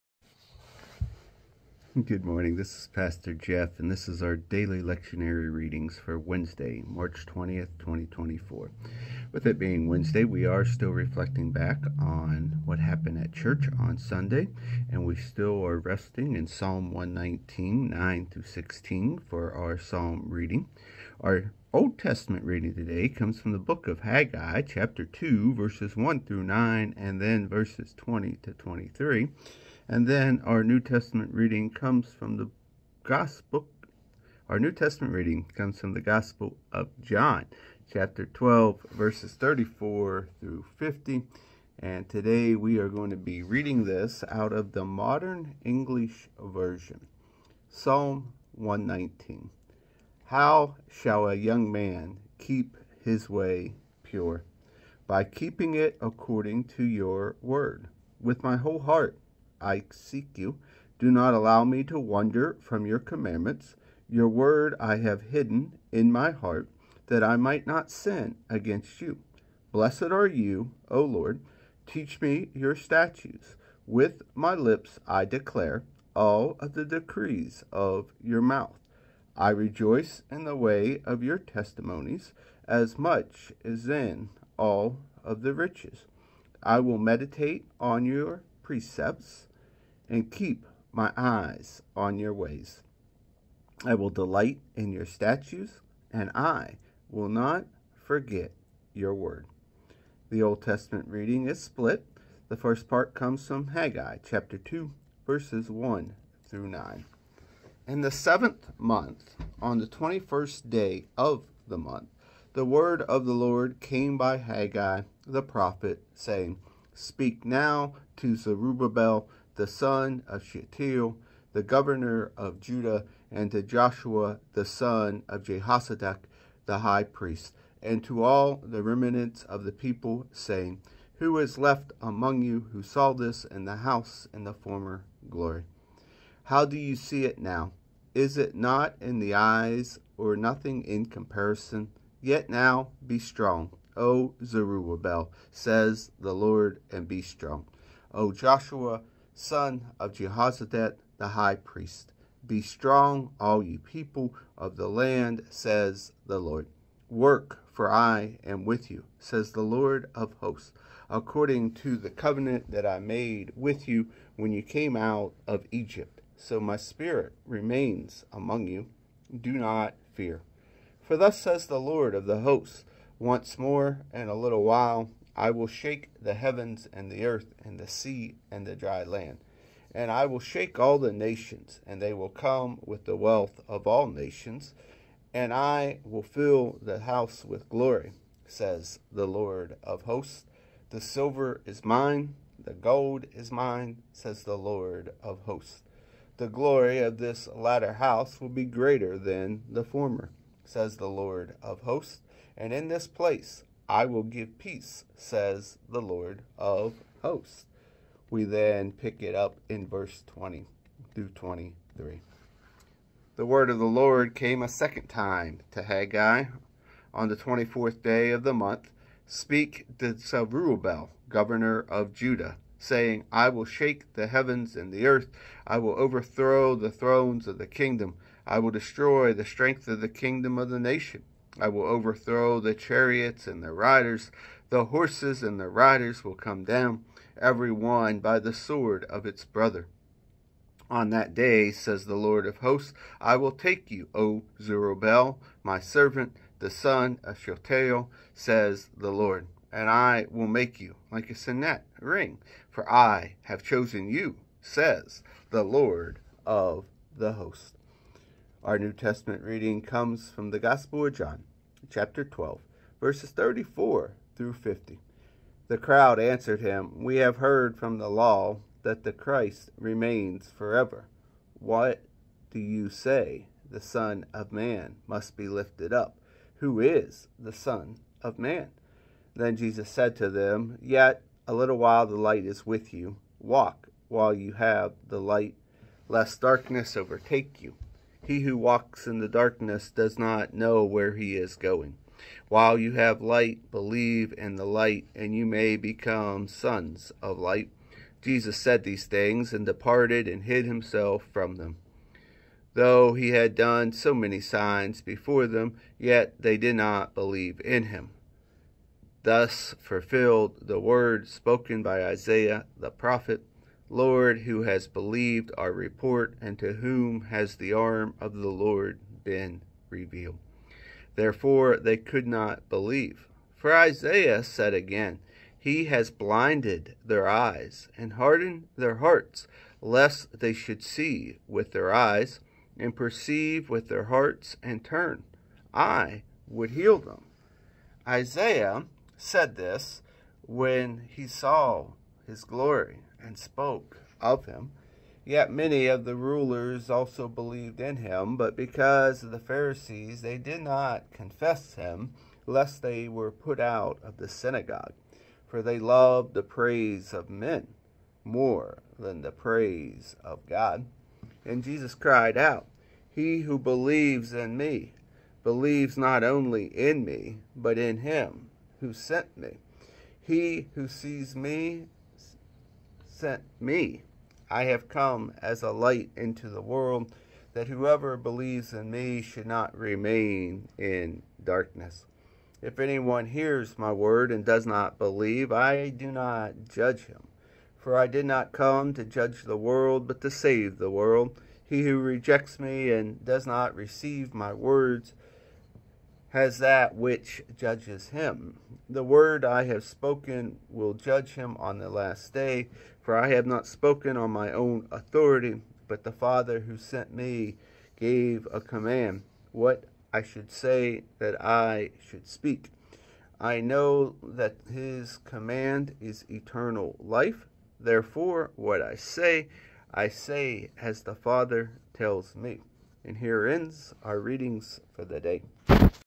Daily Readings: March 20th, 2024 (Wednesday) Time: 7 minutes and 07 seconds
Scripture readings for Wednesday, March 20, 2024 include:

Psalm Reading: Psalm 119:9-16
Old Testament Reading: Haggai 2:1-9, 20-23
Gospel Reading: John 12:34-50